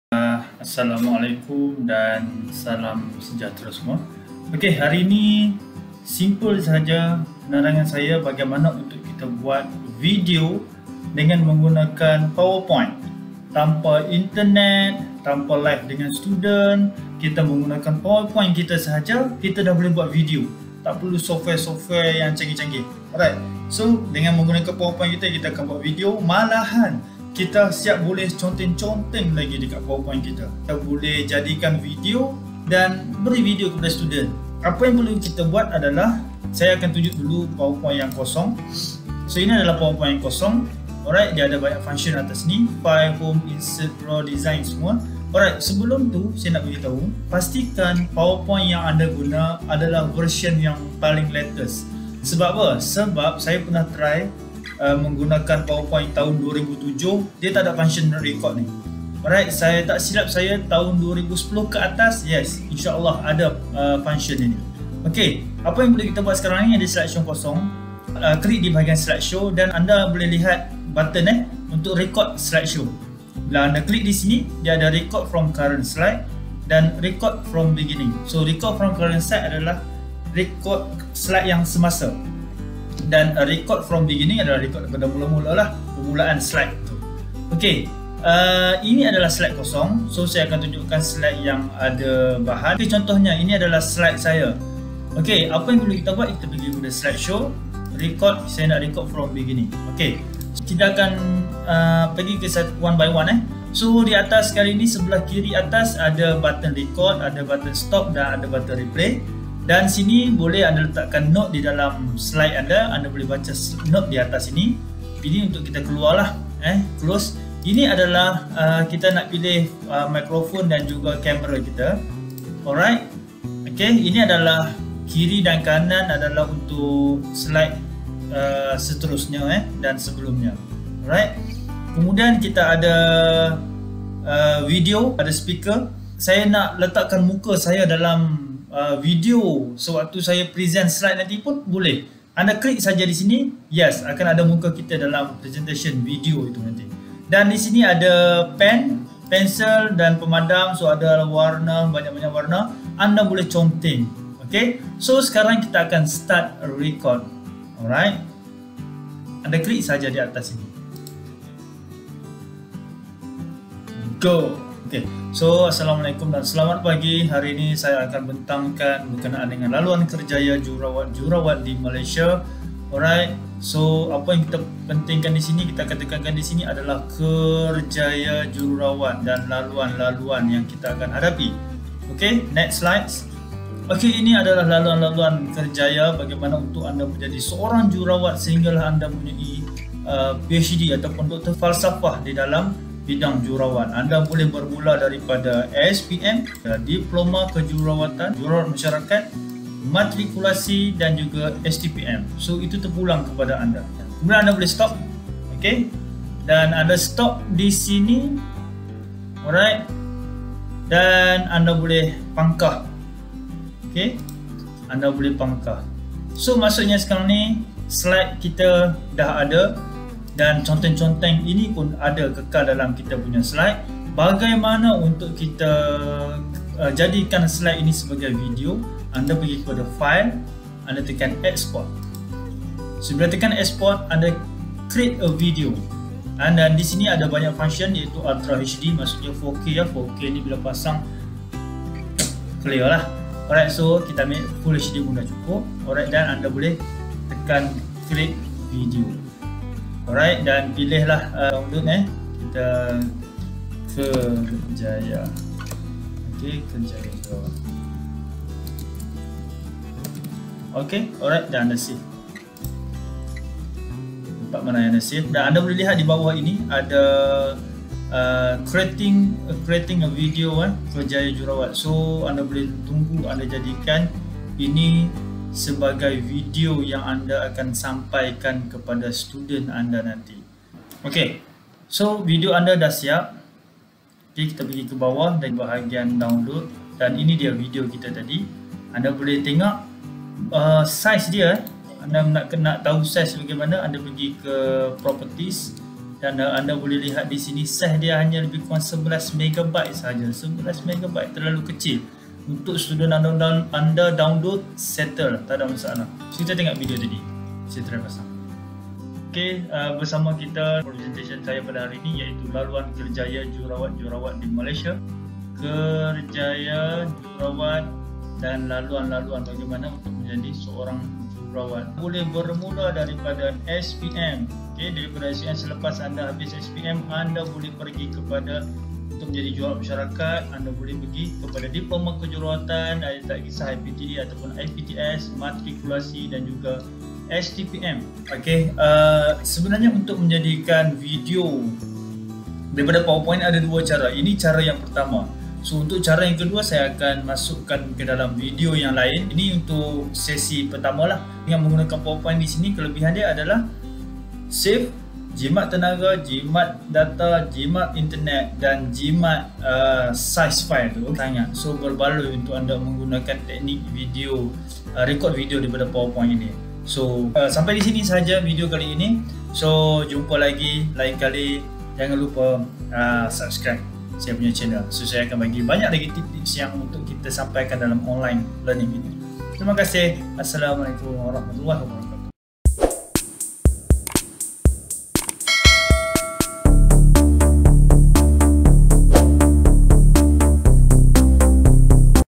Tutorial Record Video guna PowerPoint - Cara Mudah (Sesi 1)
Mungkin ramai yang tidak tahu, dengan hanya menggunakan PowerPoint, anda boleh record video pembentangan anda dan jadikan dalam bentuk video.

- Tak perlu guna internet
- Boleh ada muka dalam video
- Boleh conteng2
- Save dalam format sehingga 4k

Ini baru sesi 1, sesi kedua menyusul kemudian.